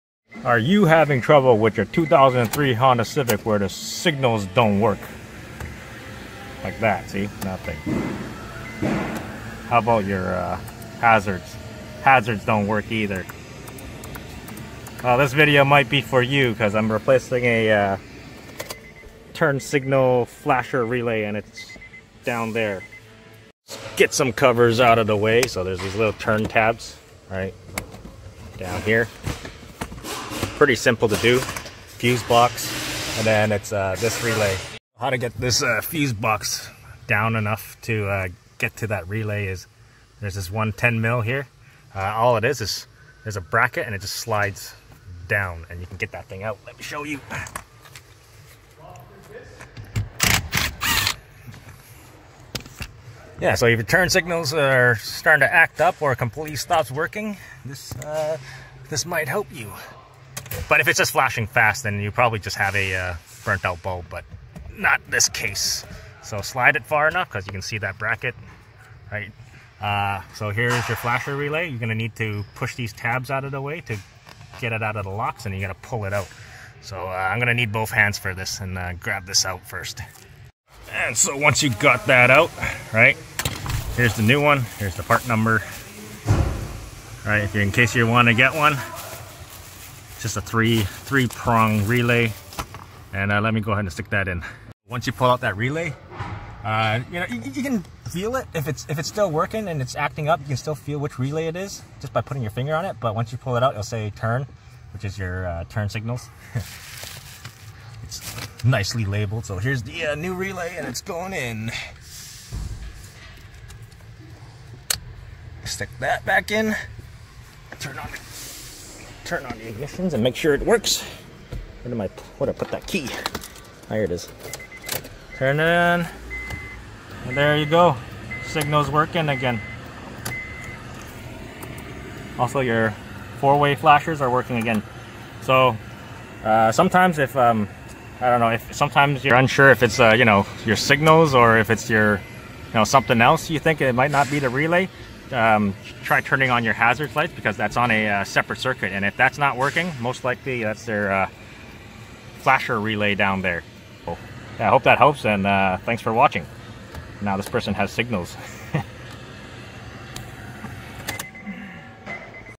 2003 Honda civic turn signals acting up and finally stopped working. Here is how you can fix it.
The Offroading Mechanic amazon store